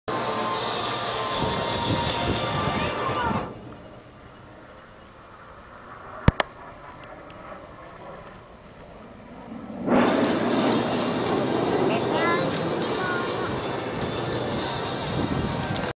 Blue Impulse at Hyakuri Sep 2009 part 5 航空自衛隊百里基地航空祭
"Blue Impulse", The aero acrobatic team of Japan Air Self-Defence Force,
they performed at Hyakuri air base festival on Sep. 13rd 2009.